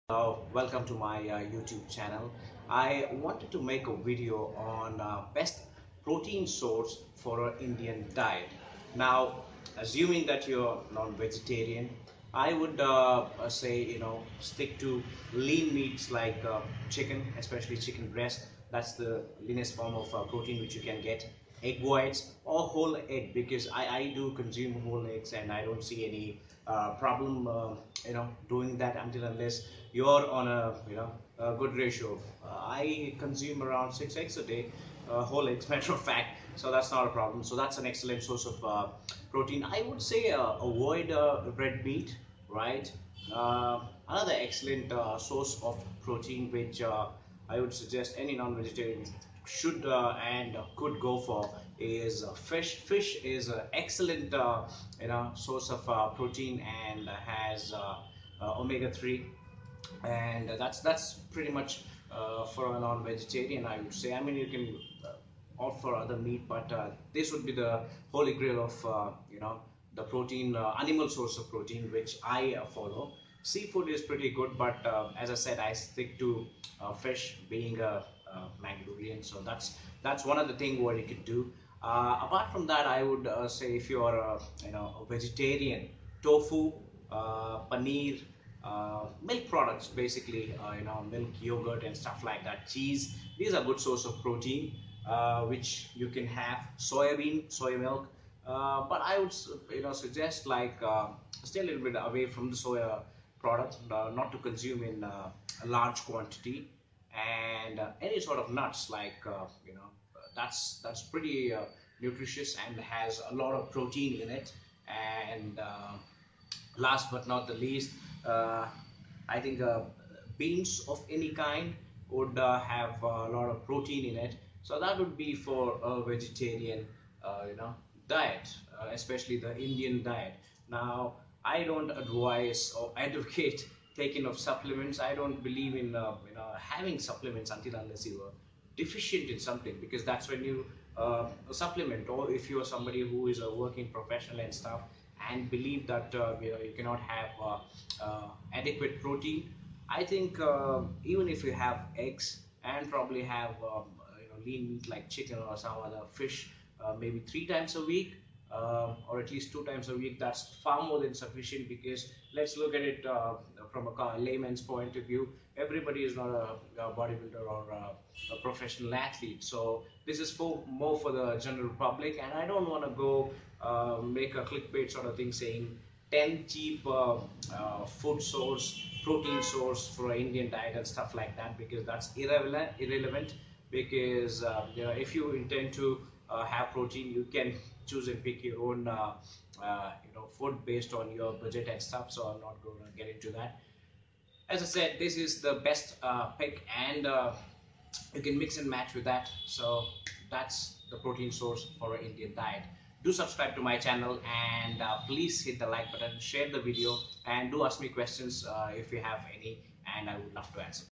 Best Protein Source for Indian Diet
I often get asked what the best Protein Source is for Indian Diet. Well the answer is neither complex nor very simple; there are varieties of food from which you can get the desired macros. What matters are your food choice and your goal, that’s the most important aspect one needs to focus on.

I am a Non-vegetarian and I like to get my protein from chicken and fish. Chicken and fish are one of the best sources of protein as they have low fat and great protein. I love eggs and I like to eat whole eggs rather than just egg whites.

When it comes to vegetarian source of protein I like paneer, tofu and cheese but I like to stay away from those foods and use them sparingly in my diet.